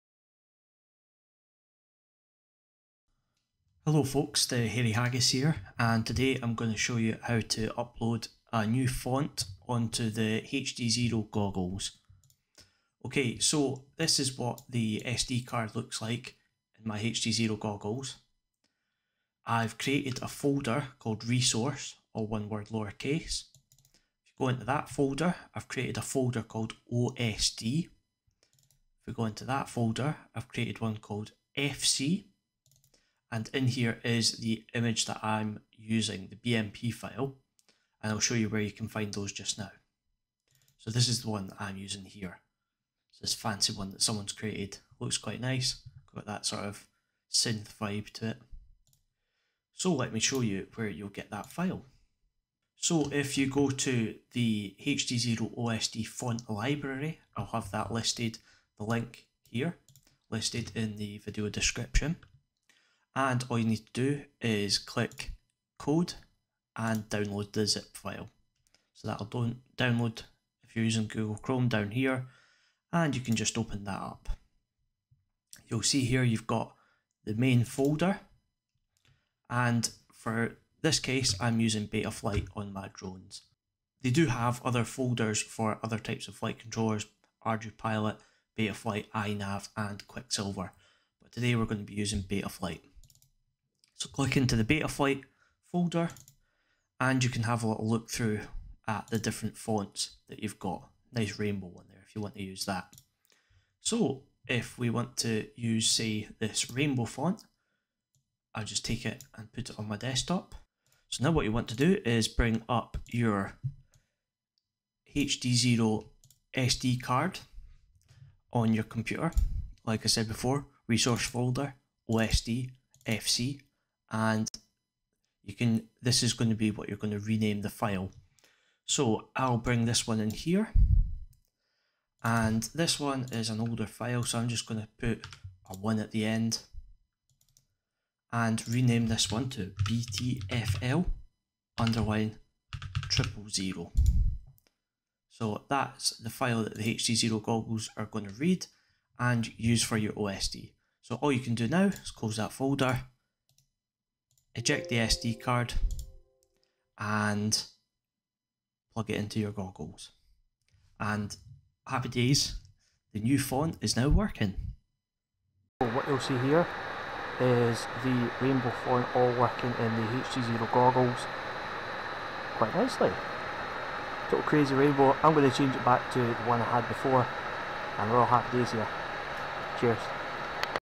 HDZero Goggles - Upload HD Fonts
I hope this video has been helpful. Thanks to kossseba in the Discord group for helping me get the fonts to work. I wanted to create this video to help others as I've not yet seen a how to.